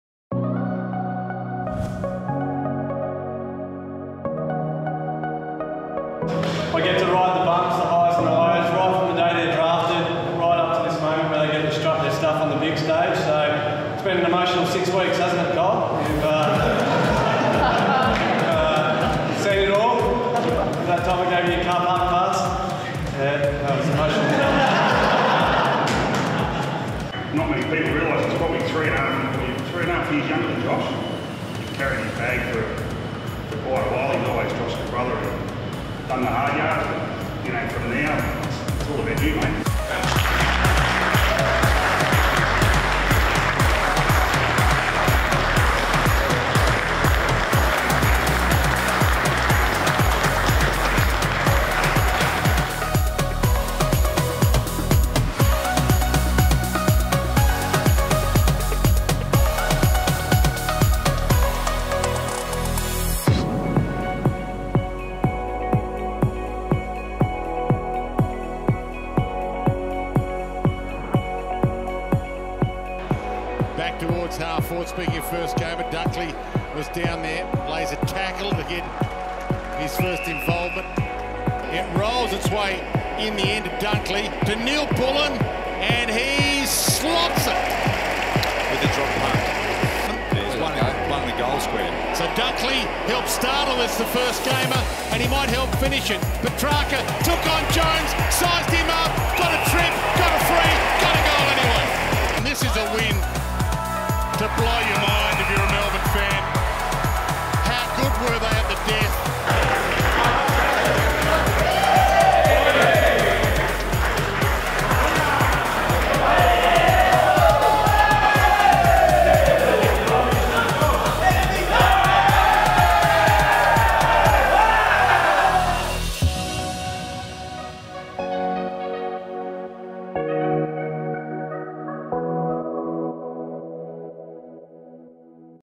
The Perfect Debut | Kyle Dunkley
Go behind the scenes as Kyle Dunkley celebrated his debut match with a Gatorade shower.